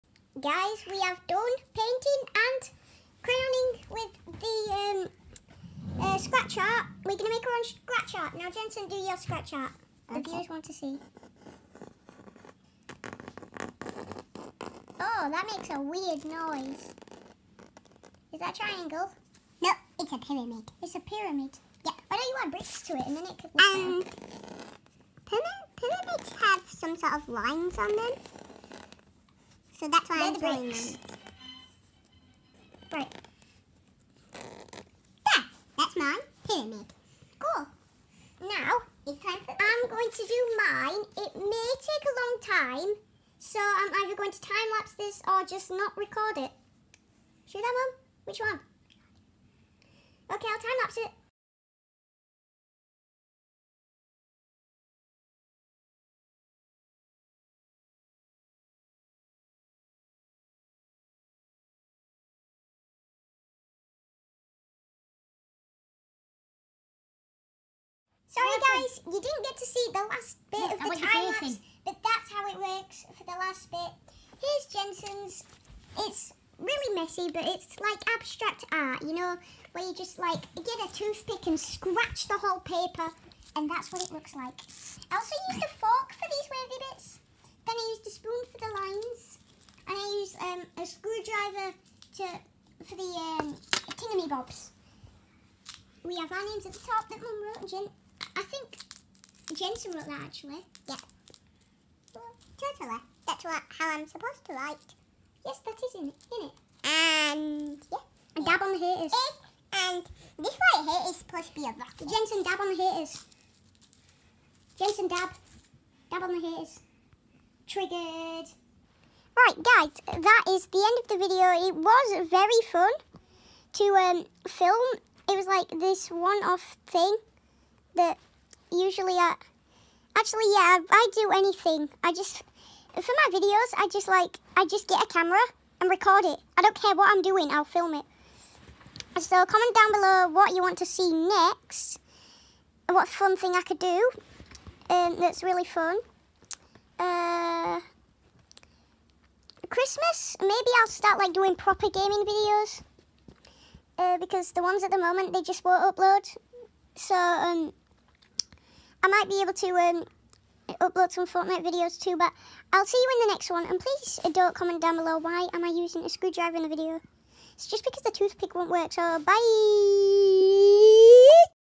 scratch art w (high voice)
in this video i try to make my very own scratch art